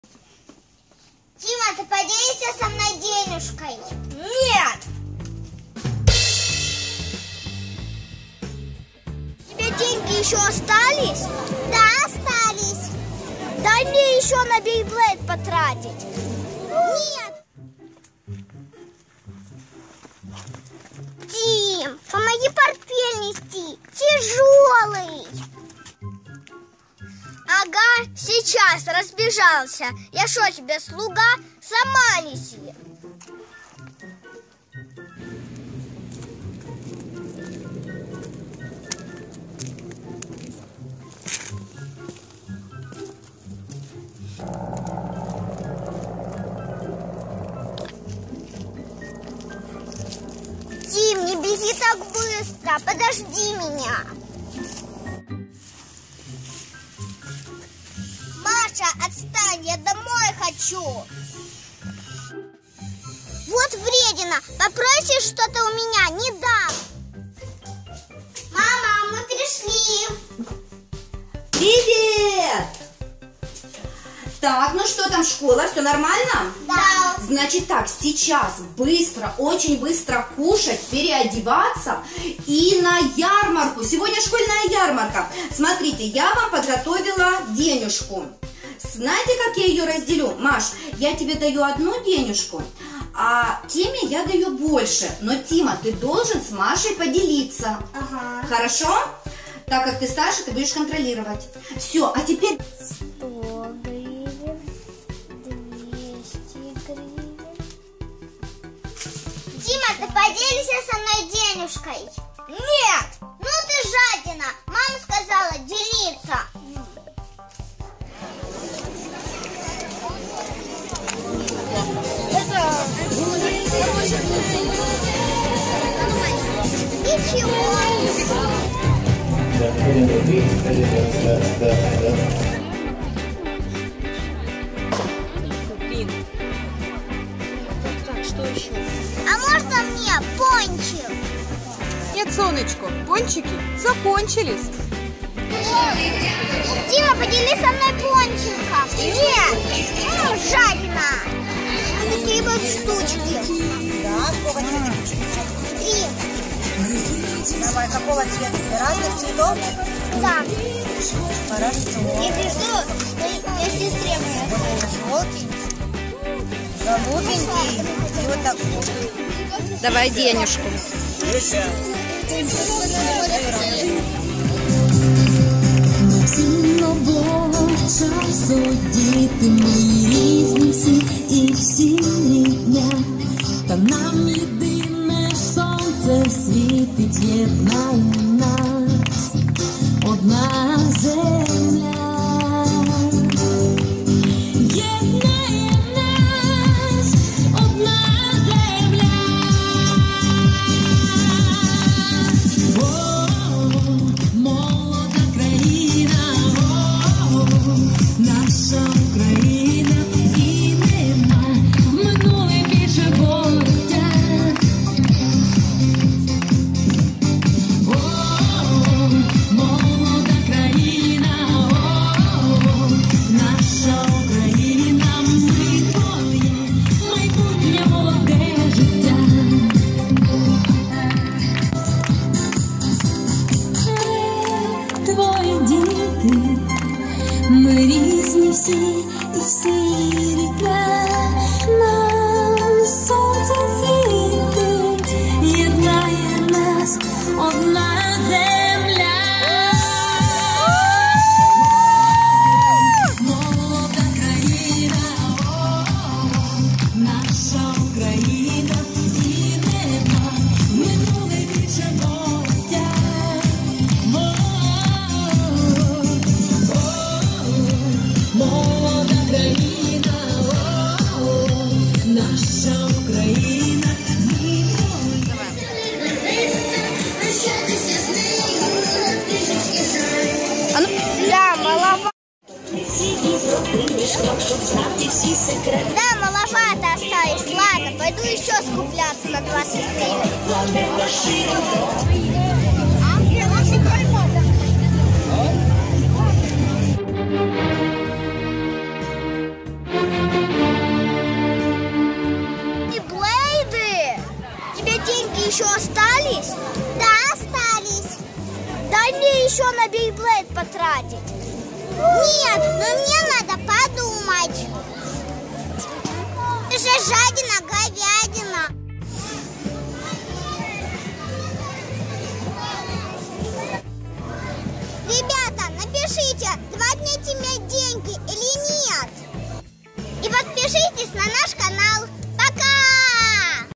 Тима ЖАДНИЧАЕТ и не делится ! Остался без бейблейд Duo Eclipse Луна и Солнце /для детей for kids
Тима и Маша идут на школьную ярмарку! Тима жадничает и не хочет делится деньгами, которые дала мама. Но Тима хочет купить бейблейд Duo Eclipse Луна и Солнце, а денег уже нет! Поможет ли ему Маша?!